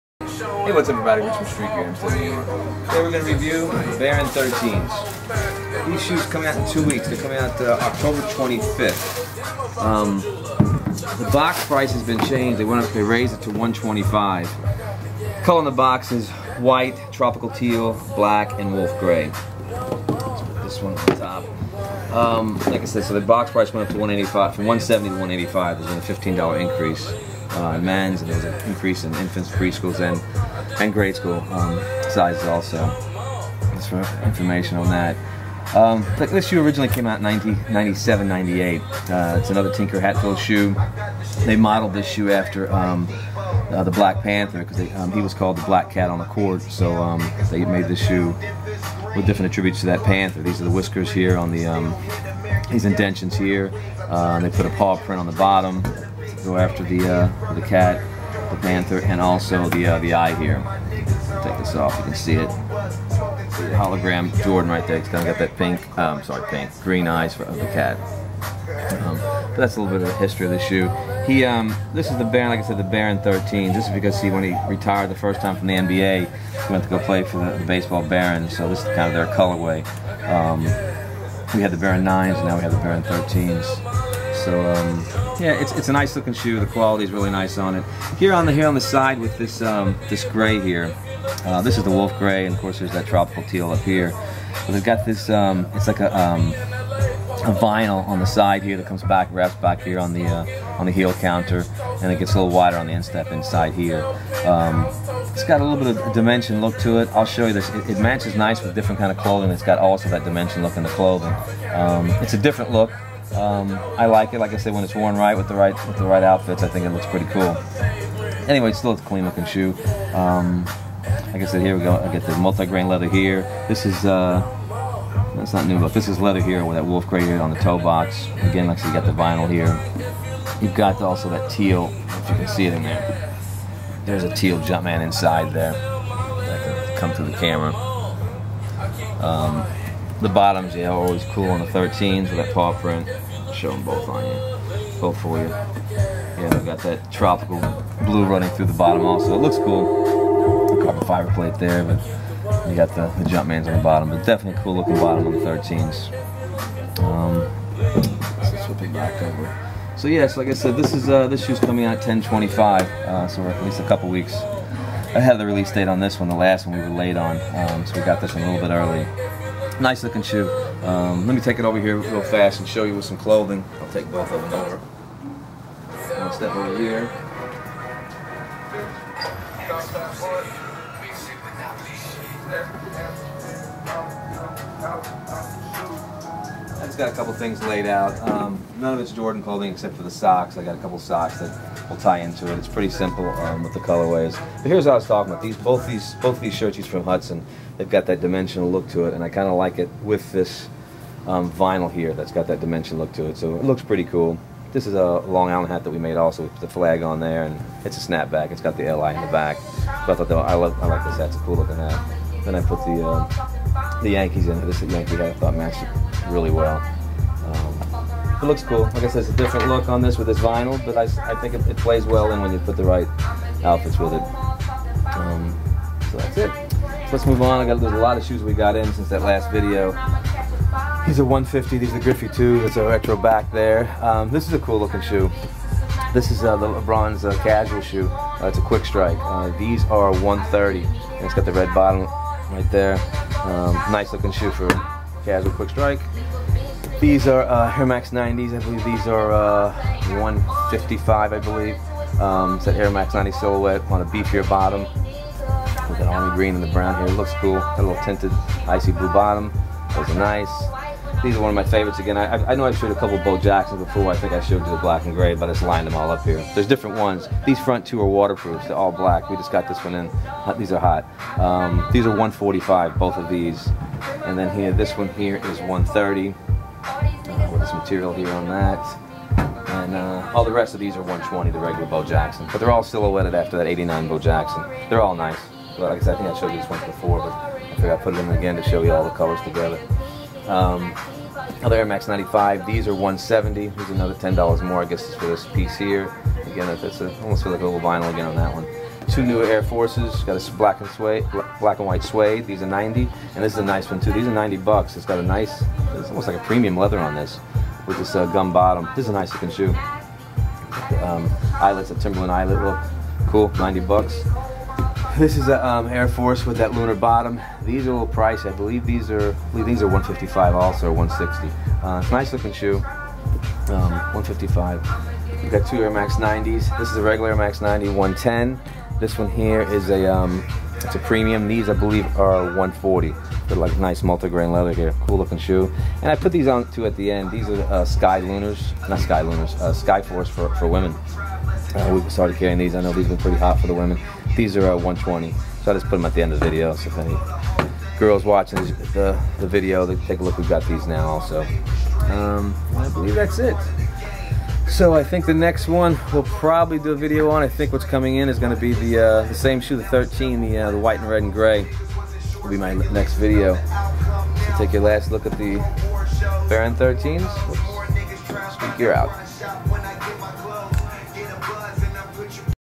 Nike Air Jordan Baron 13 - at Street Gear Hempstead, NY
Review of the Nike Air Jordan Baron 13 - at Street Gear Hempstead, NY